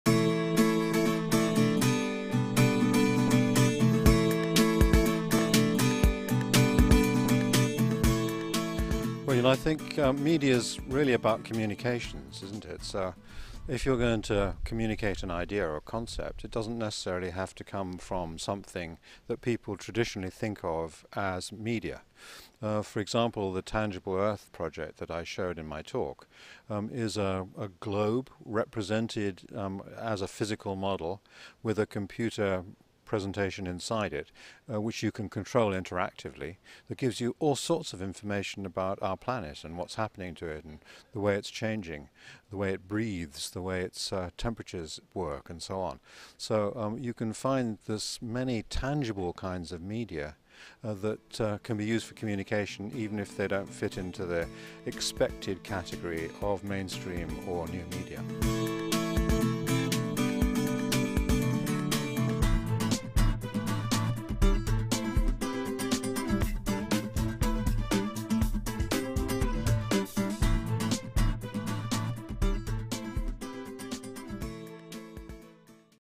Ideo Founder Ponders Media as Communications
IDEO co-founder Bill Moggridge demos "Tangible Earth Project" at Stanford's Publishing Course & challenges publishers to think beyond traditional media in strategizing for the future.